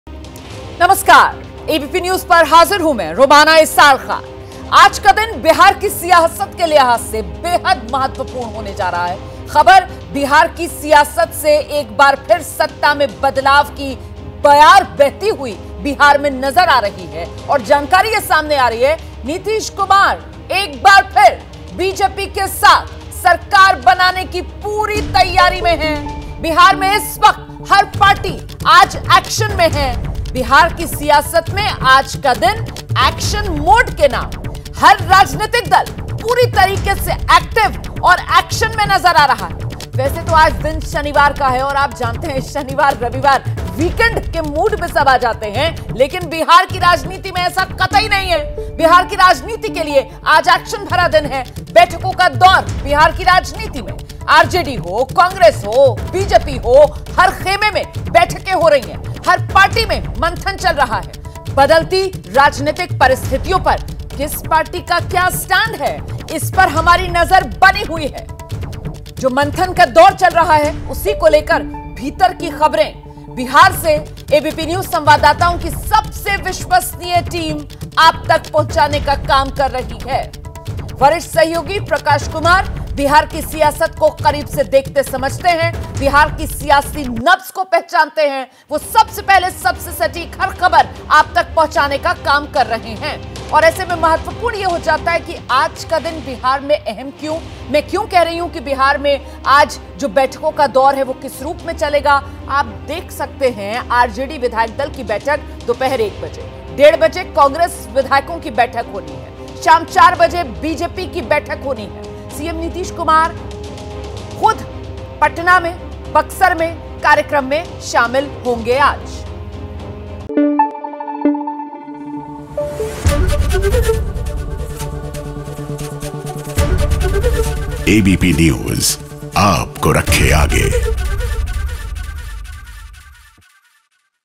Bihar की सियासत में गर्मागर्मी, एक बार फिर BJP के साथ मिलकर सरकार बनाएंगे Nitish Kumar !
बिहार के मुख्यमंत्री नीतीश कुमार एक बार फिर बीजेपी के नेतृत्व वाले एनडीए का हिस्सा हो सकते हैं. सूत्रों ने यह जानकारी दी है. नई सरकार में नीतीश कुमार मुख्यमंत्री होंगे. सरकार बनाने के बाद सबसे पहले विधानसभा अध्यक्ष का चुनाव होगा और मंत्रिमंडल में पुराने फॉर्मूले के तहत ही मंत्रियों की संख्या तय होगी.